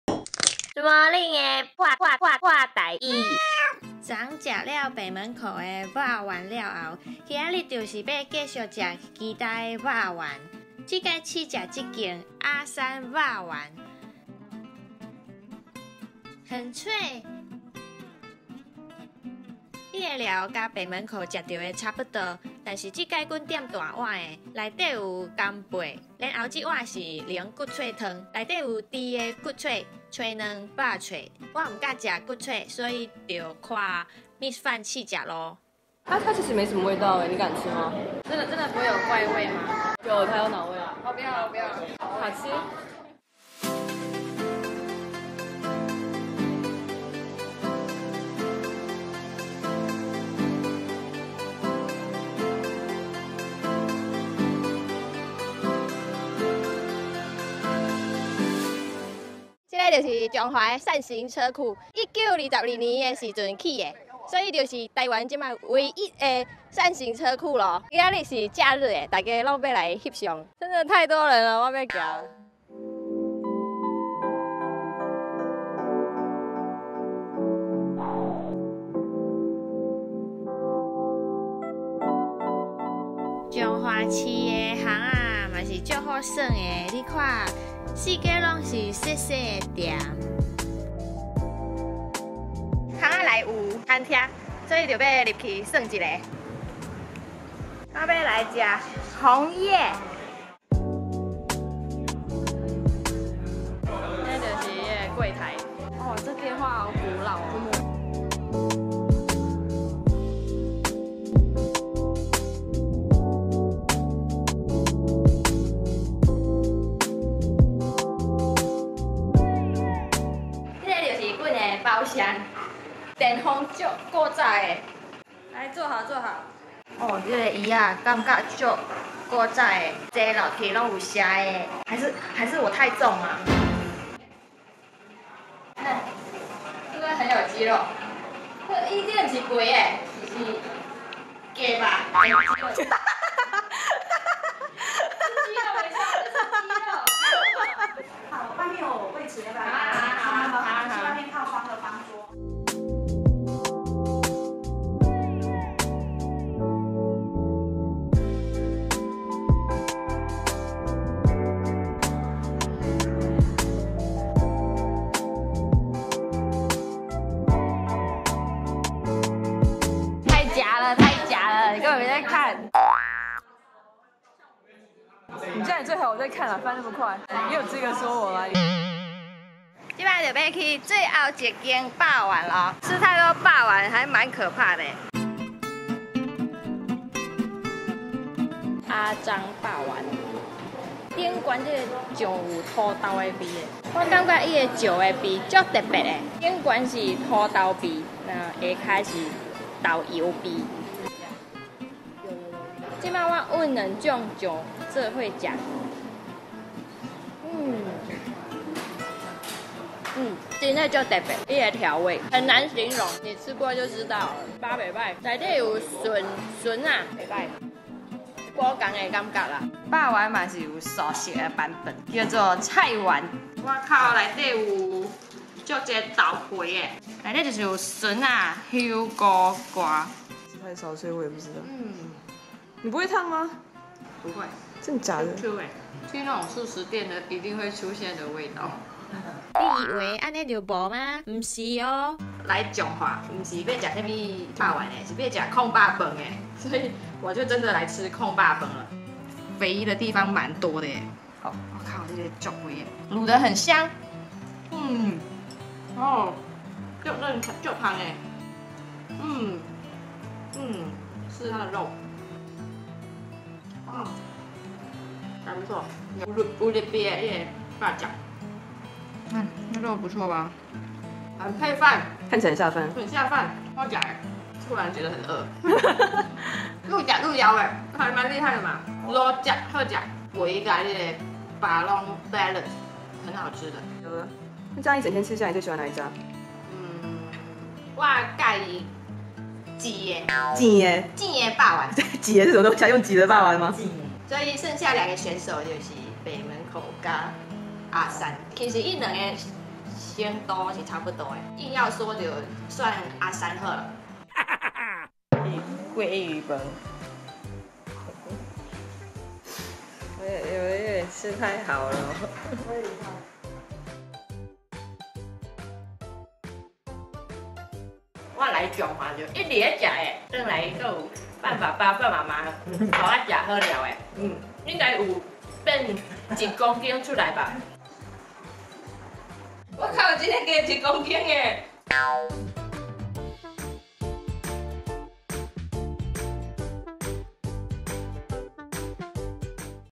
彰化不只有肉圓，爌肉飯也很猛！還有扇形車庫，紅葉大旅社，小巷子瞎逛呢～「Vlog＃50」
彰化的vlog已結束(只有兩集XD)
那時候的彰化實在太熱了，走在外頭都有點狼狽
不過這裡的美食，的確讓我變成一顆肉圓了 😓

2.